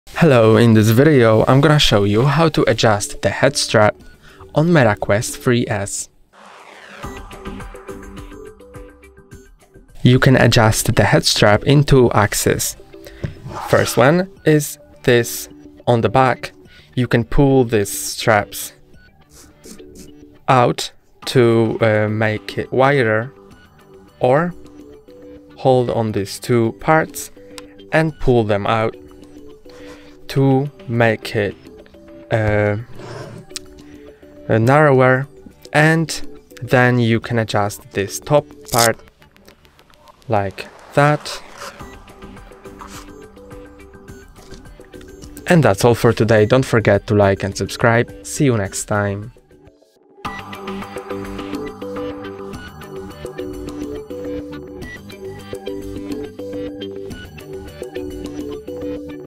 How to Adjust the Head Strap on Meta Quest 3S
In this tutorial, learn how to adjust the head strap on your Meta Quest 3S for optimal comfort and fit! Properly adjusting the head strap can enhance your VR experience by providing better stability and reducing fatigue during long sessions. Follow our step-by-step guide to customize your head strap settings for the best fit.

How do I ensure a comfortable fit for my Meta Quest 3S head strap?
How to tighten or loosen the head strap on Meta Quest 3S?
What are the best practices for adjusting the head strap on Meta Quest 3S?